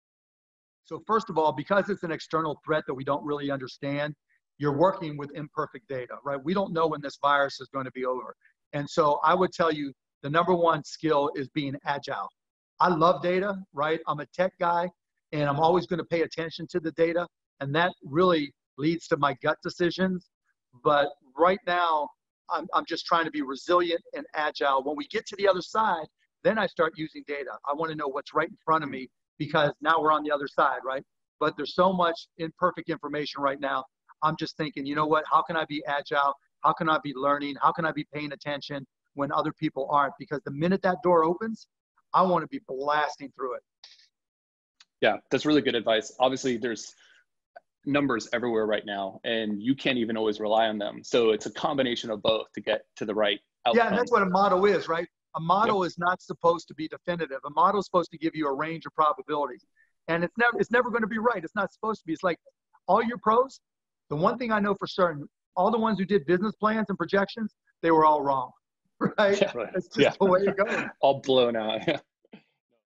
Mark Cuban: Should You Trust Data or Instincts in Business Decisions
The Housecall Pro Home Service Evening Update team welcomes Dallas Mavericks owner, Shark Tank investor, and serial entrepreneur Mark Cuban. In this video, Mark Cuban discusses whether you should be trusting your instinct or use data to make business investment decisions and what each scenario looks like.